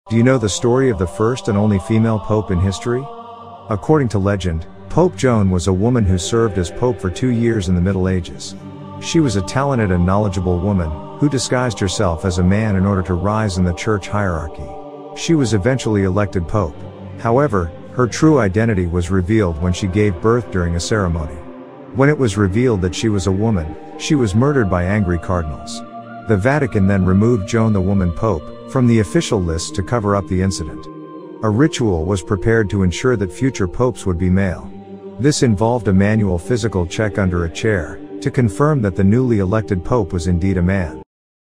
Woman Pope: A Historical Mystery!
Do you know the story of the first and only female Pope in history?

According to legend, Pope Joan was a woman who served as pope for two years in the Middle Ages.

 She was a talented and knowledgeable woman, who disguised herself as a man in order to rise in the church hierarchy.
 She was eventually elected pope. However, her true identity was revealed when she gave birth during a ceremony.
 When it was revealed that she was a woman, she was murdered by angry cardinals.

 The Vatican then removed Joan the Woman Pope from the official lists to cover up the incident.
 A ritual was prepared to ensure that future popes would be male.
 This involved a manual physical check under a chair, to confirm that the newly elected Pope was indeed a man.